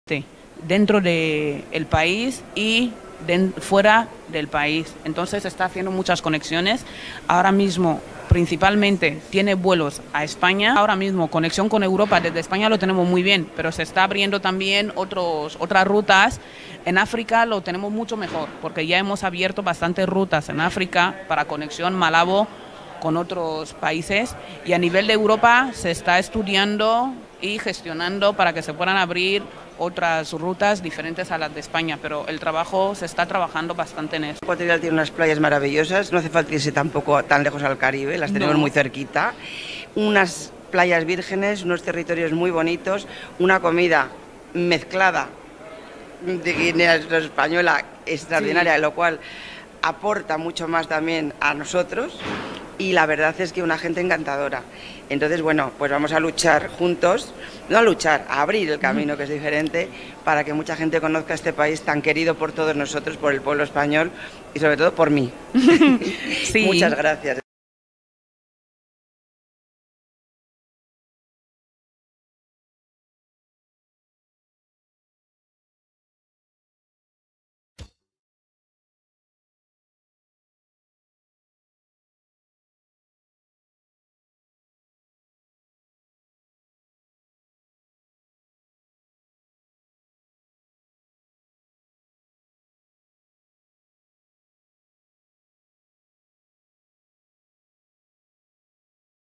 Guinea Ecuatorial un paraíso por descubrir
Guinea Ecuatorial, único país que tiene como idioma oficial el español en toda África esta creando día tras día políticas de desarrollo turístico.  Todo ello para impulsar la industria turística en el país y así convertirse en una Destinación Turística